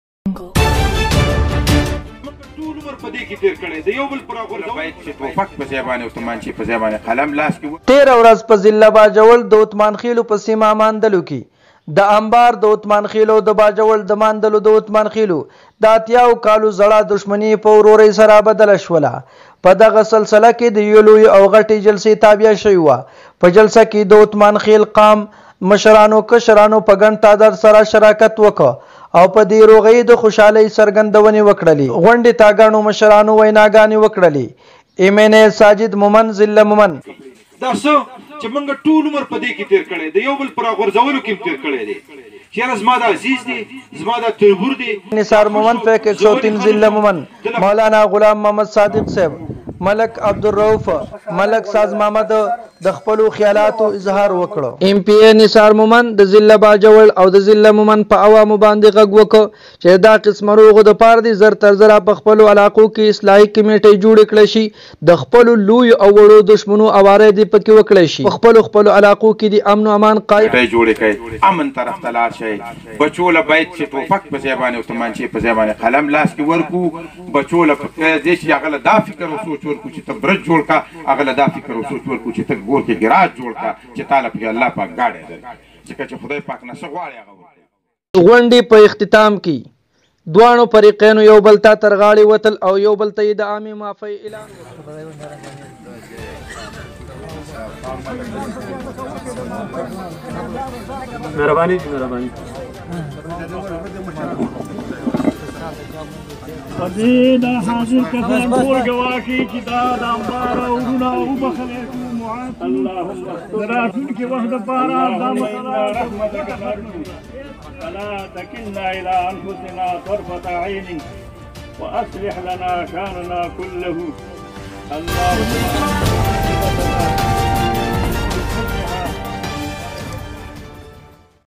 اتمان خیل بریکنگ نیوز باجوڑ علاقہ ماندل میں 80 سالہ پرانی اراضی دشمنی کا خاتمہ
اتمان خیل بریکنگ نیوز
باجوڑ علاقہ ماندل میں 80 سالہ پرانی اراضی دشمنی کا خاتمہ، فریقین آپس میں بغل گیر

تفصیلات کے مطابق علاقہ ماندل اتمان خیل میں دو فریقین کے درمیان دشمنی کے خاتمے کے سلسلے میں پر وقار تقریب کا اہتمام کیا گیا ۔ تقریب میں رکن قومی اسمبلی ساجد مہمند (ضلع مہمند) ، نثار مہمند (ضلع مہمند) ، سابقہ ایم این اے مولانا غلام صادق ، ملک فضل رازق، ملک میاں مسعود جان اور اتمان خیل قوم کے مرکزی سیکرٹری  رحمان اللہ اتمان خیل، ملک ساز محمد، حاجی گلا آمین ، میاں مسعود جان خان ، محمد رووَف اور علاقائی مشران سمیت کثیر تعداد میں لوگوں نے شرکت کی ۔ جرگہ ممبران کے کوششوں سے فریق اول حاجی بخت محمد ، ملک شاہجہان ، ملک یار خان (ساکنان امبار اتمان خیل) اور فریق دوئم ملک شہزادہ ، تاج محمد (ساکنان ماندل اتمان خیل) کے درمیان 80 سال قبل جنم لینے ولی اراضی دشمنی دوستی میں بدل گئی ۔ اس موقع پر مشران نے دونوں فریقوں کو بغلگیر کروادیا گیا ۔ اور دونوں فریقوں نے آئندہ کیلئے بھائیوں کی طرح رہنے کا اعلان کیا ۔ تقریب سے خطاب کرتے ہوئے رکن قومی اسمبلی ساجد مہمند (ضلع مہمند) ، نثار محمد (ضلع مہمند) ، سابقہ ایم این اے مولانا غلام صادق، ملک میاں مسعود جان اور اتمان خیل قوم کے مرکزی سیکرٹری اطلاعات ونشریات  رحمان اللہ اتمان خیل نے اور دیگر مقرر رہنما نے کہاکہ تنازعات کسی بھی مسئلے کا حل نہیں ہے اور ہم سب کو چاہیے کہ آپس کے تنازعات باہمی جرگوں اور مذاکرات سے حل کریں ۔ انہوں نے دونوں فریقین کو مبارکباد دی ۔